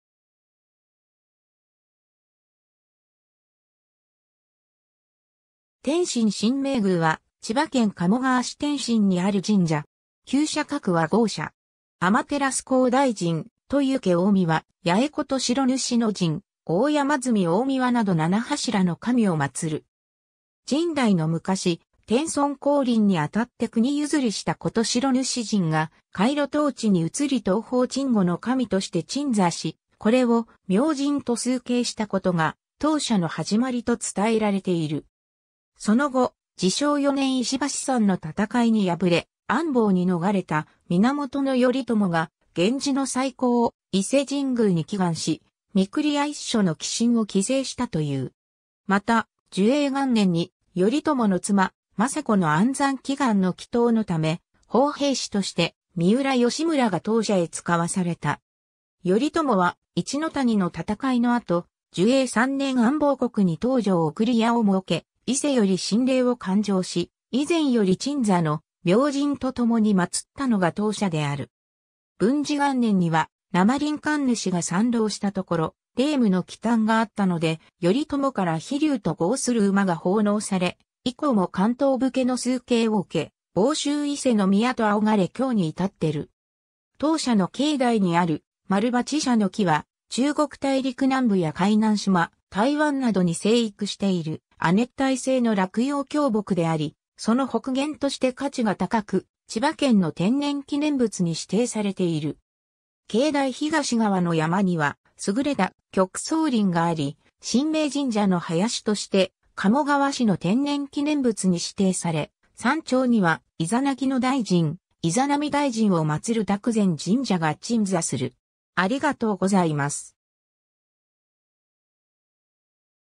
天津神明宮
天津神明宮（あまつしんめいぐう）は、千葉県鴨川市天津（安房国長狭郡）にある神社。
 天照皇大神・豊受大神・八重事代主神・大山祇大神など七柱の神を祀る。
 神代の昔、天孫降臨に当って国譲りした事代主神が海路当地に移り東方鎮護の神として鎮座し、これを『庤明神』と崇敬したことが当社の始まりと伝えられている。
 その後、治承4年（1180年）石橋山の戦いに敗れ安房に逃れた源頼朝が源氏の再興を伊勢神宮に祈願し、御厨一処の寄進を祈誓したという。
また、寿永元年（1182年）に、頼朝の妻政子の安産祈願の祈祷のため、奉幣使として三浦義村が当社へ遣わされた。
頼朝は一ノ谷の戦いの後、寿永3年（1184年）安房国に東条御厨（白浜御厨、阿摩津御厨）を設け、伊勢より神霊を勧請し以前より鎮座の『庤明神』とともに祀ったのが当社である。
文治元年（1185年）には生倫神主が参籠したところ、霊夢の奇端があったので頼朝から飛龍と号する馬が奉納され、以降も関東武家の崇敬を受け「房州伊勢の宮」と仰がれ今日に至ってる。
 当社の境内にある「まるばちしゃの木」は、中国大陸南部や海南島、台湾などに生育している亜熱帯性の落葉喬木であり、その北限として価値が高く千葉県の天然記念物に指定されている。
境内東側の山にはすぐれた極相林があり「神明神社の林」として鴨川市の天然記念物に指定され、山頂には伊邪那岐大神・伊邪那美大神を祀る諾冉神社が鎮座する。